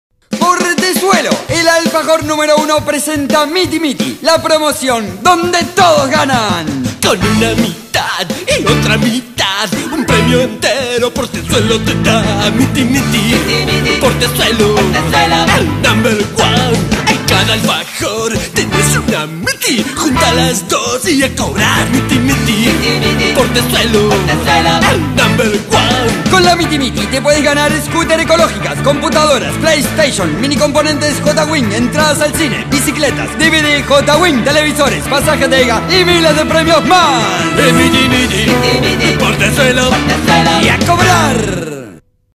Publicidad Alfajores Portezuelo Uruguay (2004)
Publicidad transmitida por Canal 4,Canal 10,Teledoce Uruguay en 2004